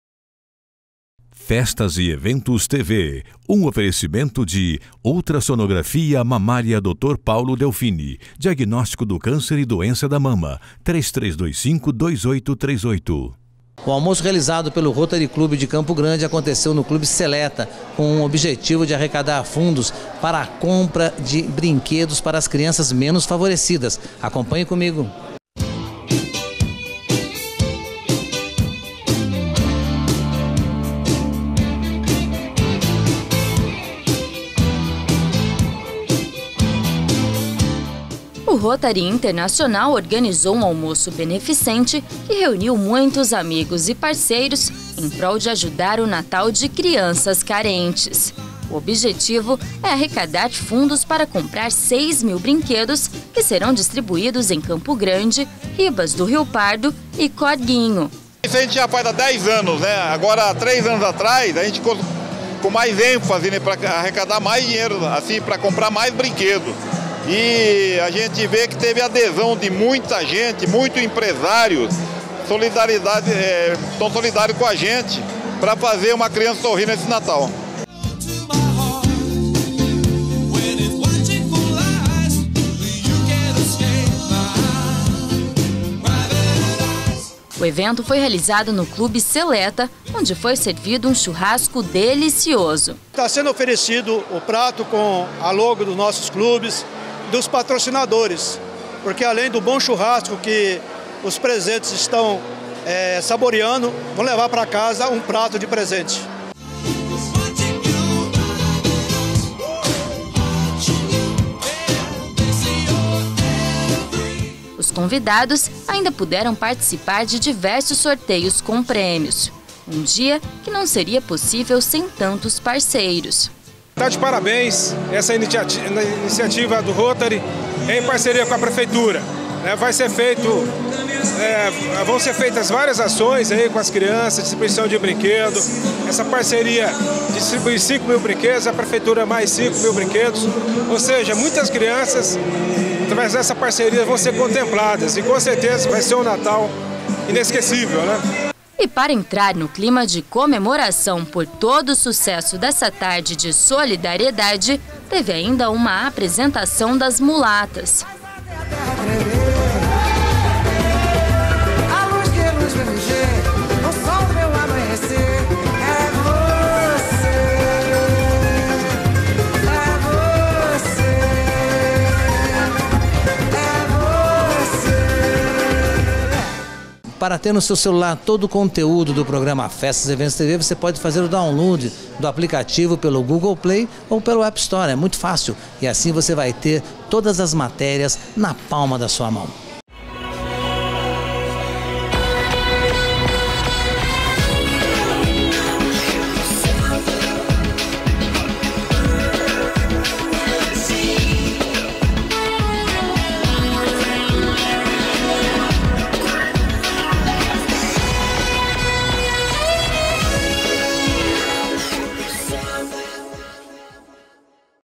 Almoço Beneficente do distrito 4470 Rotary Internacional
O almoço realizado pelo Rotary de Campo Grande, aconteceu no clube SELETA com o objetivo de arrecadar fundos para a compra de brinquedos para as crianças menos favorecidas da cidade.